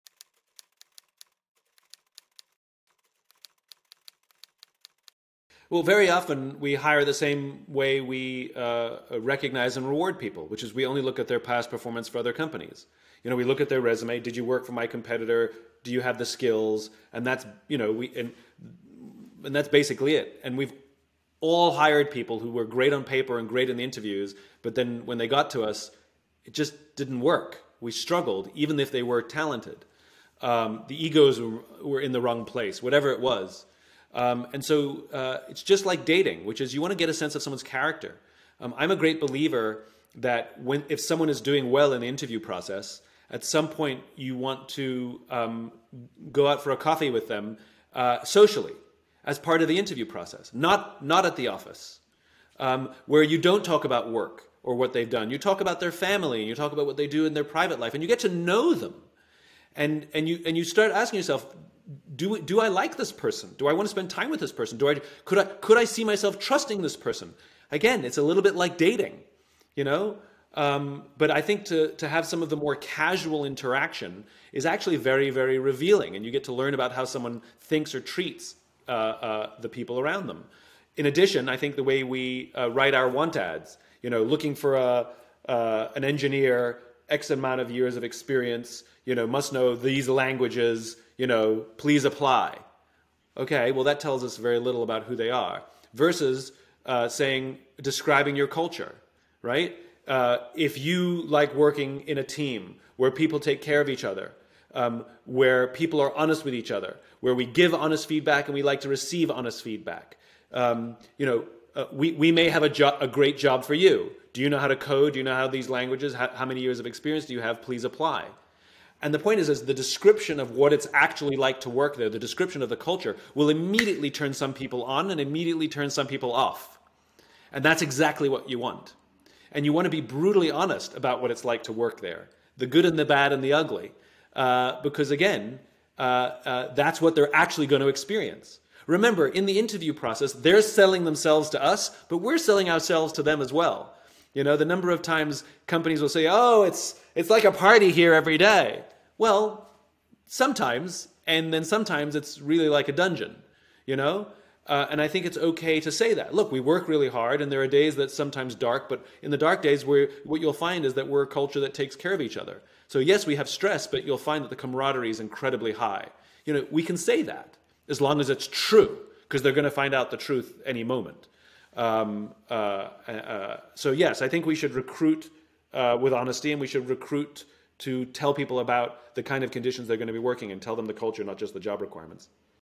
A Better Way to Hire | Simon Sinek
How do we make sure we hire the best people for a job? Get a sense of their character, not just their qualifications.